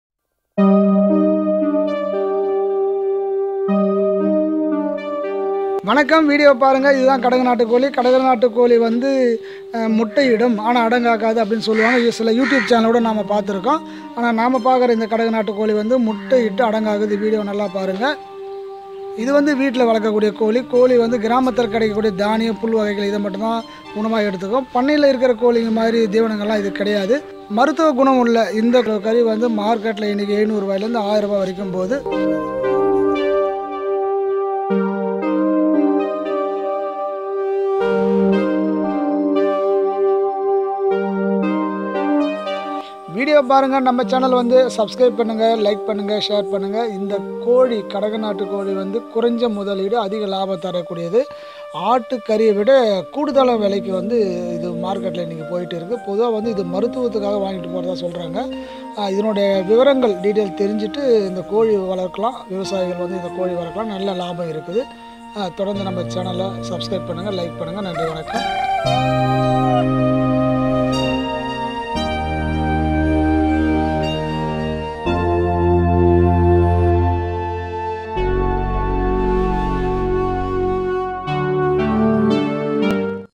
கடகுநாட்டு கோழி வளர்ப்பு.
இந்த வகை கோழி குறைந்த முதலீட்டில் அதிகம் லாபம் தரக்கூடியது தொழில்
அனைவரும் இந்த வகை கோழி வளர்க்கவும்.